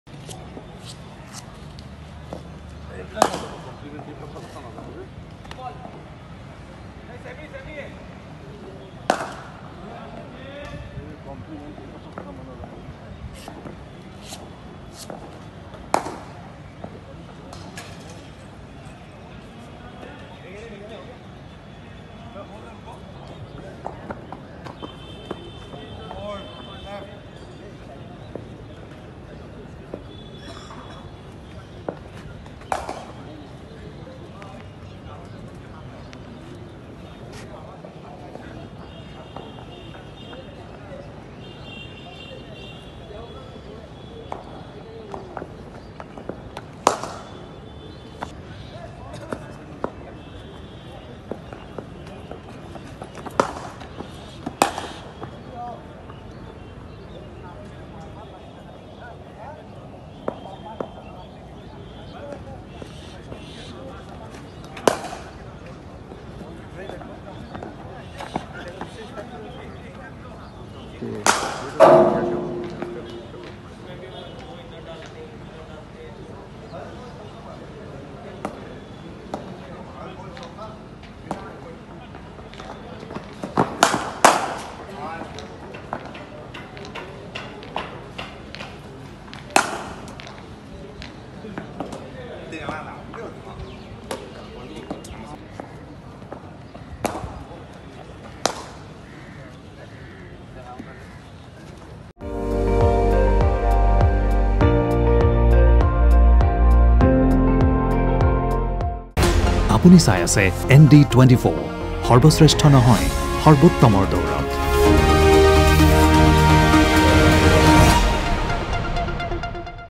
বৰ্ষাপাৰাত চলিল টীম ইণ্ডিয়াৰ নেট প্ৰেক্টিচ
দেওবাৰে বৰ্ষাপাৰাত T-20ৰ হাইভল্টেজ মেচ ৷ বিয়লি বৰ্ষাপাৰাত চলিল টীম ইণ্ডিয়াৰ নেট প্ৰেক্টিচ ৷